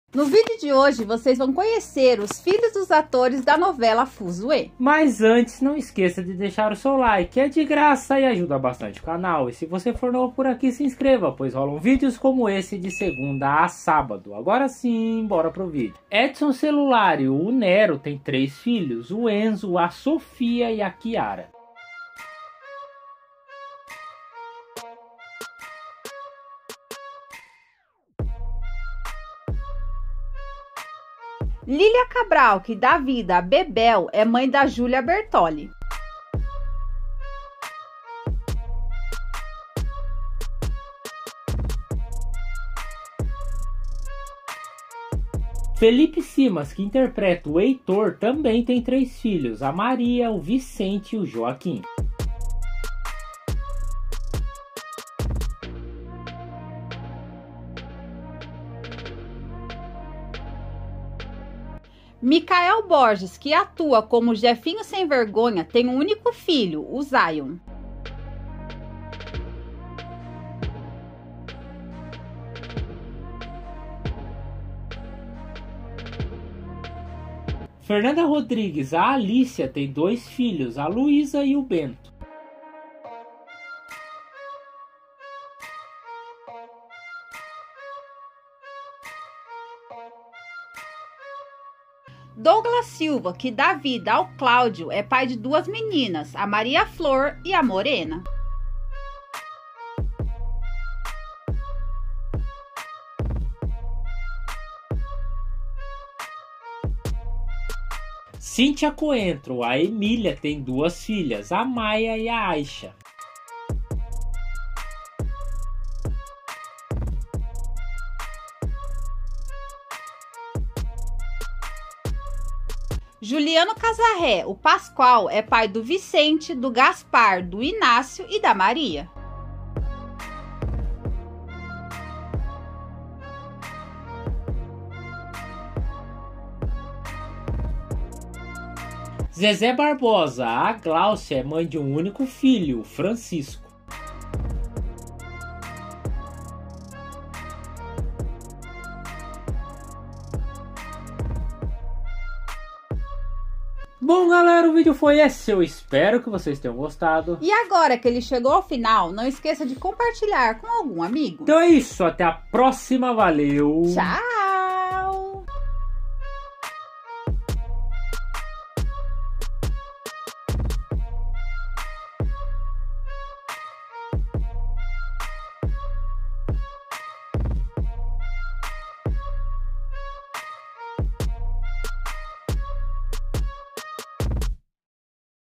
FILHOS dos ATORES da NOVELA FUZUÊ!
Hoje vocês irão ver quem são os filhos dos atores da novela Fuzuê!